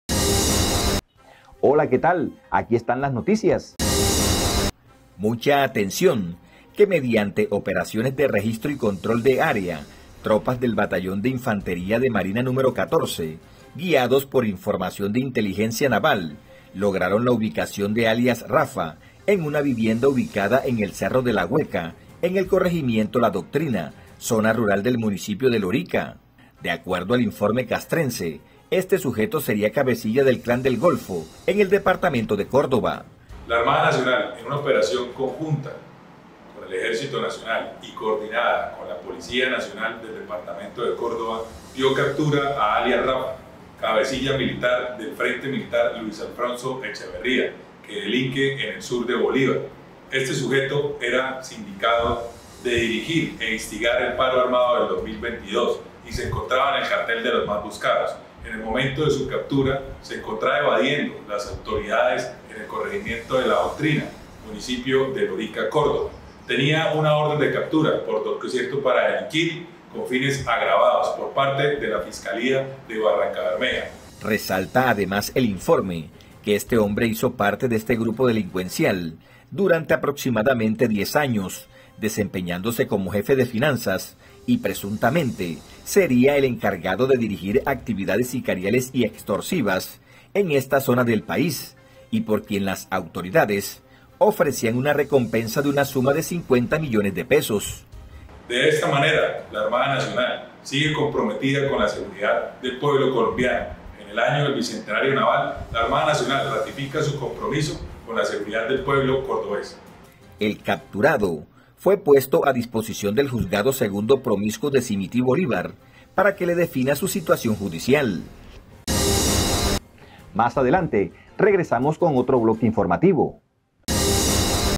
Capturado Alias “Rafa”, presunto cabecilla del “Clan Del Golfo”
Noticias de Santa Marta y Magdalena

La captura se dio en cumplimiento de una orden de allanamiento en su contra emitida por la Fiscalía de Lorica, por los presuntos delitos de concierto para delinquir agravado.